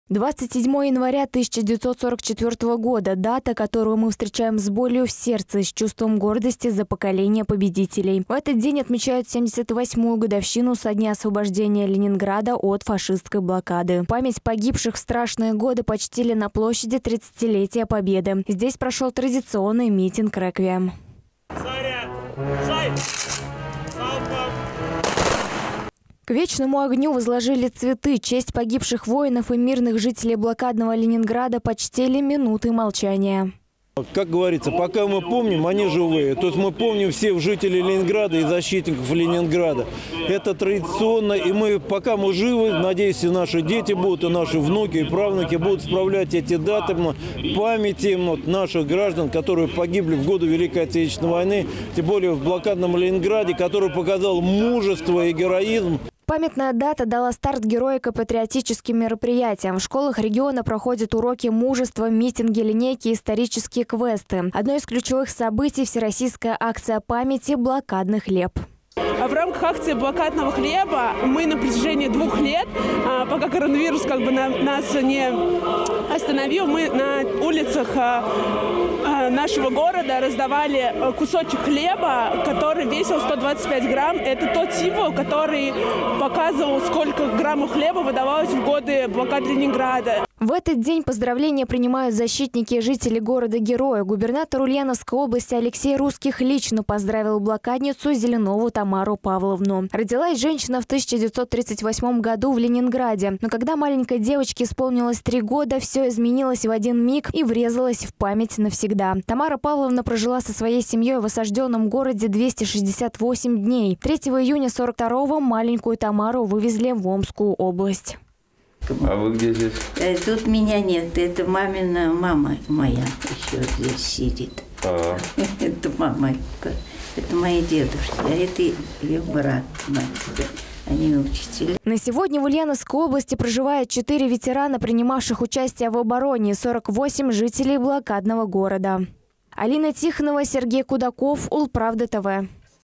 В Ульяновске почтили память погибших в блокаду Ленинграда
78 лет исполнилось со дня снятия блокады Ленинграда. От голода и обстрелов в городе-герое погибло около миллиона жителей. Жертв бомбежек и массового голода вспомнили по всей России.